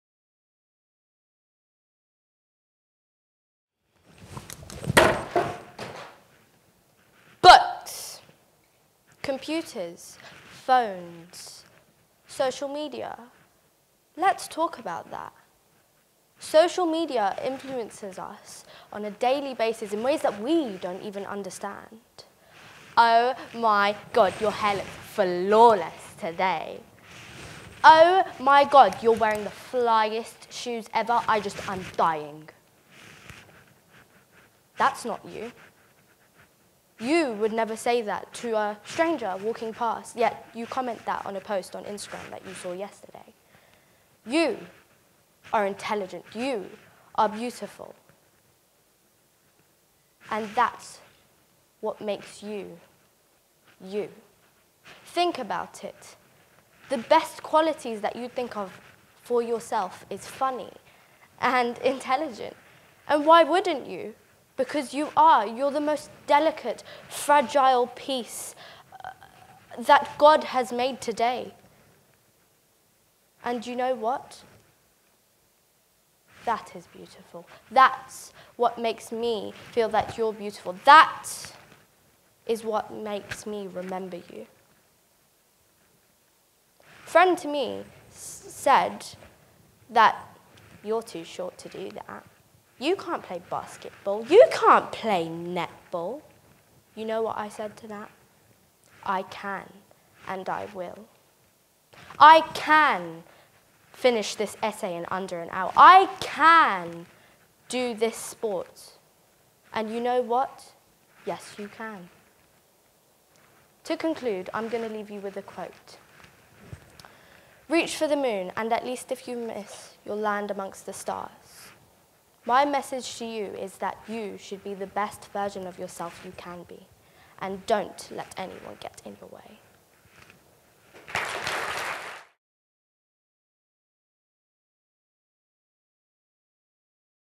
Janna Metwally, Third, Jack Petchey’s “Speak Out” Challenge! Ealing Regional Final 2017-18
Janna Metwally of William Perkin CofE High School took third place at the 2017-18 “Speak Out” Challenge! Ealing Regional Final with a speech entitled ‘Social Pressures’